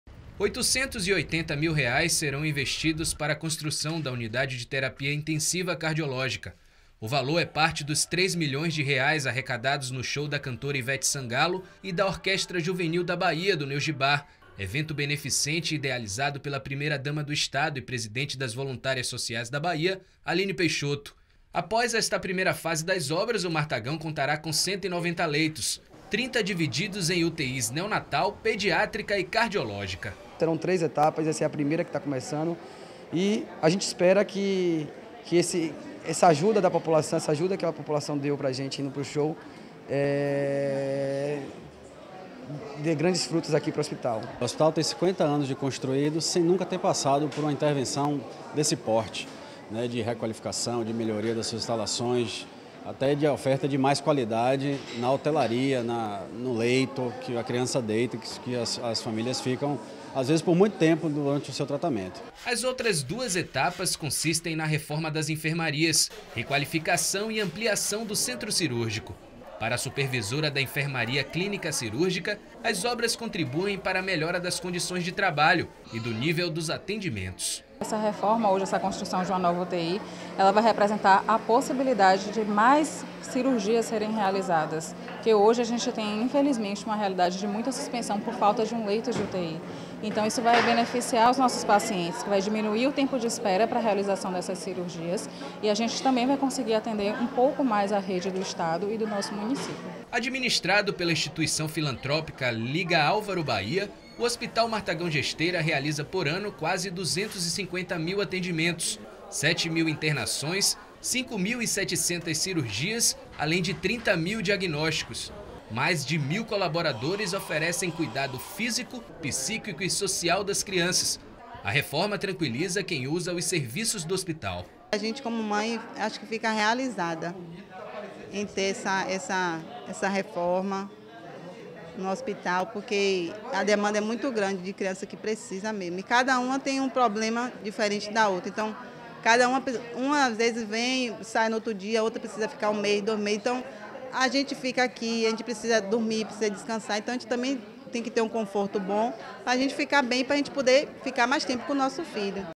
Reforma do Hospital Martagão Gesteira tem obras iniciadas
As obras de requalificação do Hospital Martagão Gesteira, em Salvador, foram iniciadas esta semana, visando garantir melhores condições de atendimento ao público infantil do estado. Ao todo, R$ 880 mil serão investidos nesta primeira etapa, que consiste na construção da Unidade de Terapia Intensiva (UTI) cardiológica em uma área da unidade que estava desativada.

O valor é parte dos R$ 3 milhões arrecadados no show beneficente da cantora Ivete Sangalo e da Orquestra Juvenil da Bahia (Neojiba), realizado com o propósito de angariar fundos para a reforma do hospital. Idealizado pela primeira-dama do Estado e presidente das Voluntárias Sociais da Bahia (VSBA), Aline Peixoto, o evento ocorreu em dezembro do ano passado, na Arena Fonte Nova, reunindo cerca de 25 mil pessoas que abraçaram a campanha.

Após a conclusão da primeira etapa das obras, que teve início na segunda-feira (4), o Martagão vai passar a contar com 190 leitos, sendo 30 divididos igualmente em UTIs neonatal, pediátrica e cardiológica. Cada uma delas terá seus respectivos equipamentos e farmácias satélite.